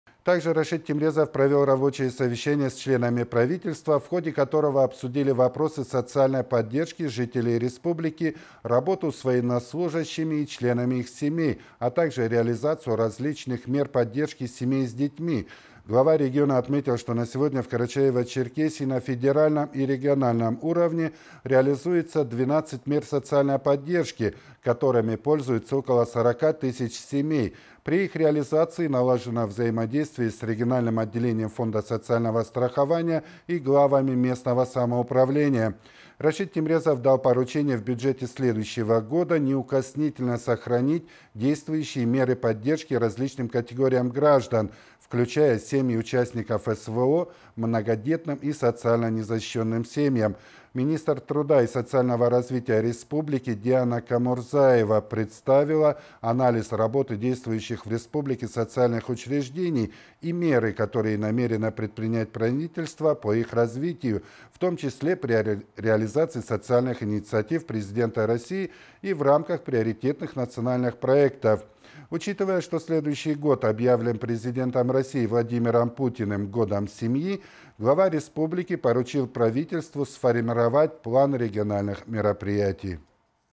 Рашид Темрезов поручил правительству сформировать план региональных мероприятий в рамках Года семьи
Глава Карачаево-Черкесии Рашид Темрезов провёл рабочее совещание с членами регионального правительства. Внимание было уделено вопросам социальной поддержки жителей республики. Так, в КЧР реализуются 12 мер оказания федеральной и региональной помощи некоторым категориям граждан, в первую очередь военнослужащим, членам их семей и семьям с детьми. Продолжится их неукоснительное исполнение и в следующем году.
«Учитывая, что следующий год объявлен Президентом России Владимиром Путиным Годом семьи, поручил Правительству сформировать план региональных мероприятий в рамках Года, который бы отражал не только событийный контекст, но и носил практический характер, включая поддержку образовательных учреждений, организаций здравоохранения и культуры, которые являются центрами семейного досуга, оздоровления и обучения», - отметил важность предпринимаемых мер Рашид Темрезов